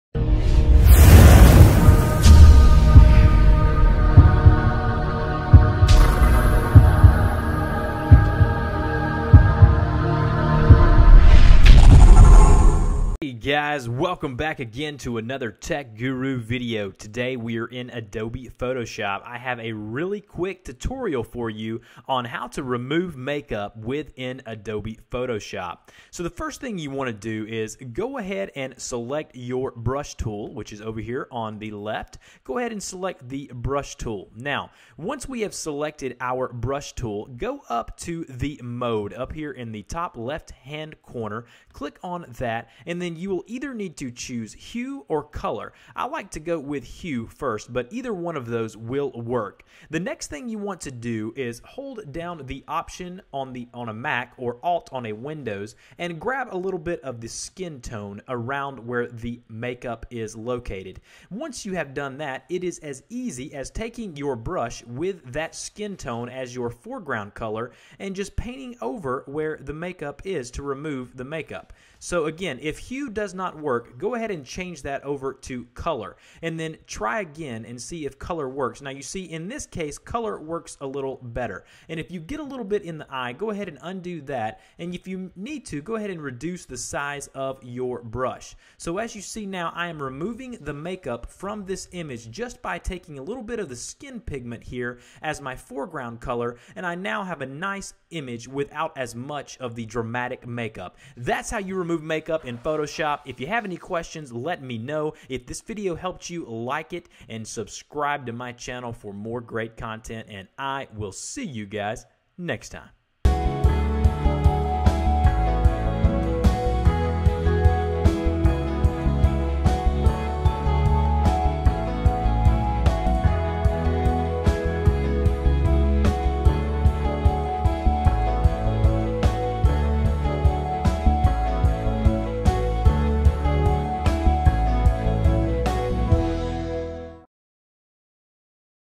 Photoshop Tutorial- How To Remove Makeup In Photoshop
In this video tutorial I show you how to remove Makeup in photoshop very easily and quickly!

1.) Select your brush tool
2.) Change the mode to either Hue OR Color it will depend on the color and intensity of the makeup.
3.) Next press and hold the alt (windows) option (mac) keys and select some skin tone around the eye.
4.) Take your paint brush and paint over the makeup with the foreground color being the skin tone that you just selected.

dmporter74